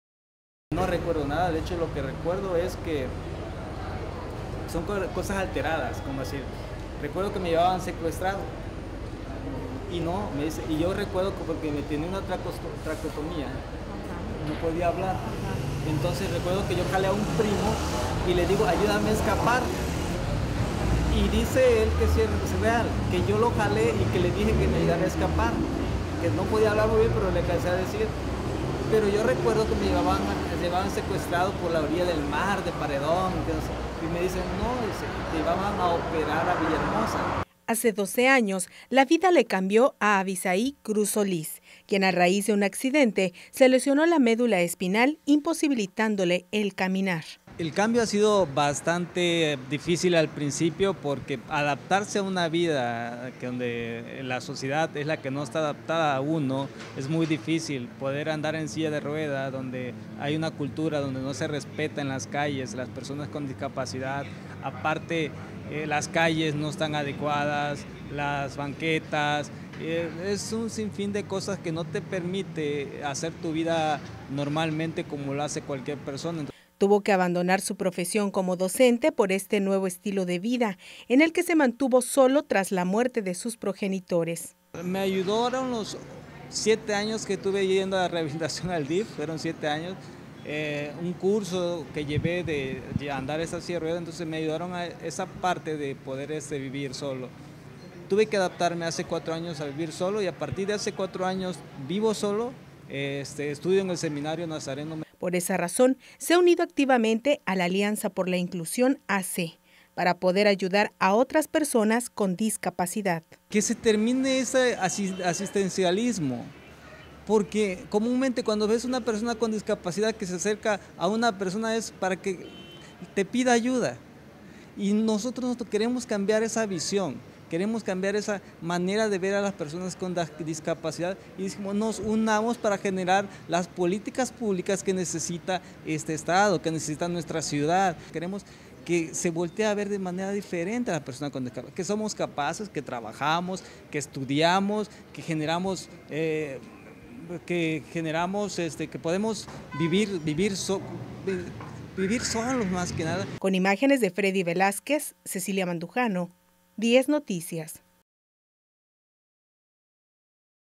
Abisaí abona a mejorar la calidad de la vida de personas con discapacidad
Hace 12 años, la vida le cambió a Abisaí Cruz Solís, quien a raíz de un accidente se lesionó la medula espinal, imposibilitándole el caminar.

Tuvo que abandonar su profesión como docente por este nuevo estilo de vida, en el que se mantuvo sólo tras la muerte de sus progenitores.